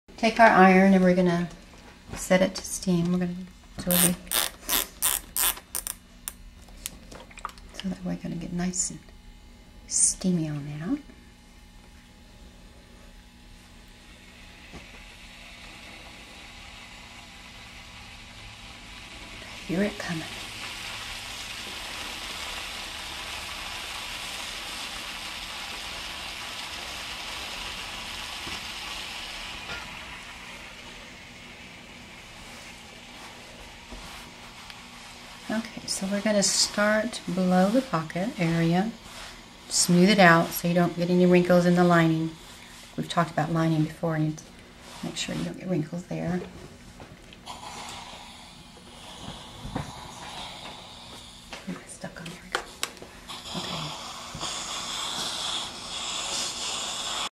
How to Iron a Suit : Iron Settings for Ironing a Suit
The right temperature for ironing. Learn how to adjust iron setting while ironing a men's suit in this free clothing care video from a wardrobe professional.

Expert: Joyce Danielson
Bio: Joyce Danielson has worked as a wardrobe assistant on such network television shows as Inspector Mom for Lifetime Television.
Filmmaker: john carstarphen